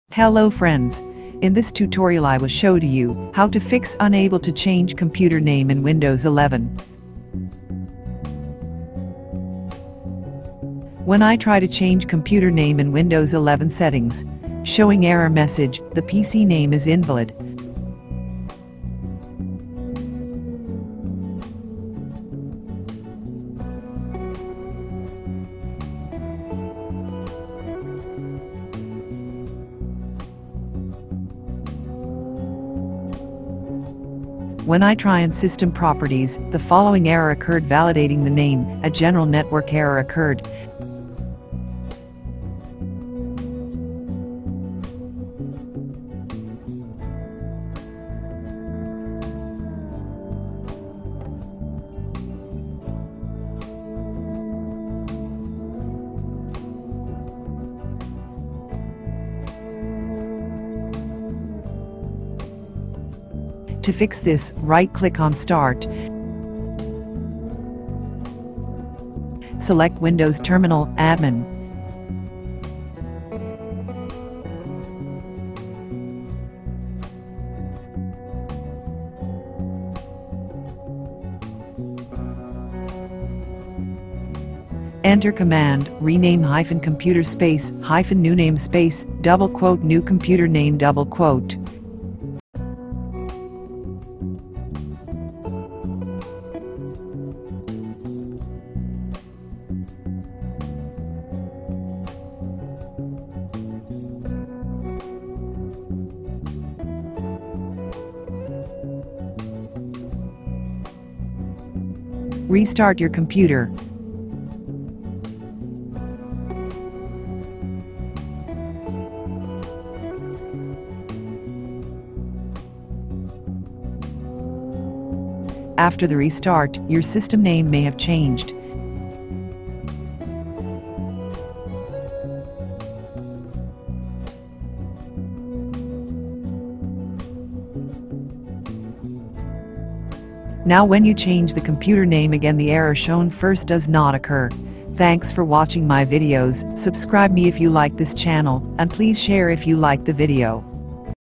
How to fix "The PC name is invalid" error when changing your PC Name on Windows 11
How to fix Unable to Change computer name in Windows 11.
How to Fix the following error occurred validating the name "PC Name", A General network error occurred.

👉How to move existing Windows 11 / Windows 10 from your old hard drive to SSD without new windows installation